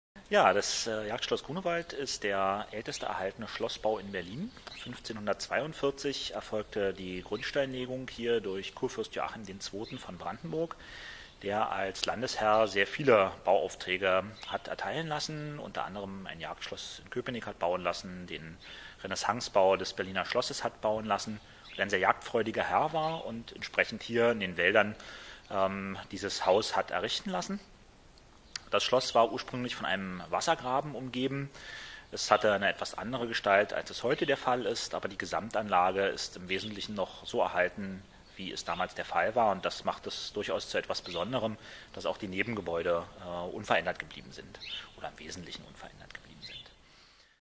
Die Grundsteinlegung des Jagdschlosses Grunewald
Das Jagdschloss Grunewald ist das älteste noch erhaltene Schloss in Berlin. Die Grundsteinlegung erfolgte 1542. Björn Ahlhelm vom Schlossmanagement Jagdschloss Grunewald erklärt, dass das Schloss ursprünglich eine etwas andere äußere Gestalt hatte als heute.